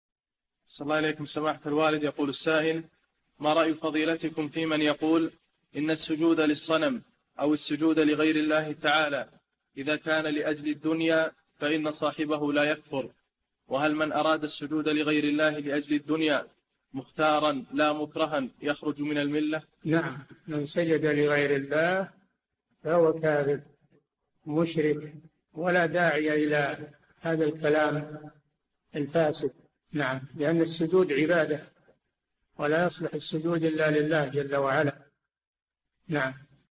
[1787- 4717] هل السجود لغير الله لأجل الدنيا يعتبر كفراً لأن هناك من يقول لا يعد كفراً؟
[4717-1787] ما رأي فضيلتكم فيمن يقول إن السجود للصنم أو السجود لغير الله تعالى إذا كان لأجل الدنيا فإن صاحبه لا يكفر، وهل من أراد السجود لغير الله لأجل الدنيا مختارا لا مكرها يخرج من الملة؟
الجَوَابُ: نعم، من سجد لغير الله فهو كافر مشرك ولا داعي إلى هذا الكلام الفاسد؛ لأن السجود عبادة، ولا يصلح السجود إلا لله جل وعلا.
السيرة الذاتية :
للاشتراك في قناة الشيخ صالح الفوزان
لمشاهدة حلقات فتاوى على الهواء كاملة

للاستفسار والإقتراحات والدعم
رئيس الفريق
لمشروع كبار العلماء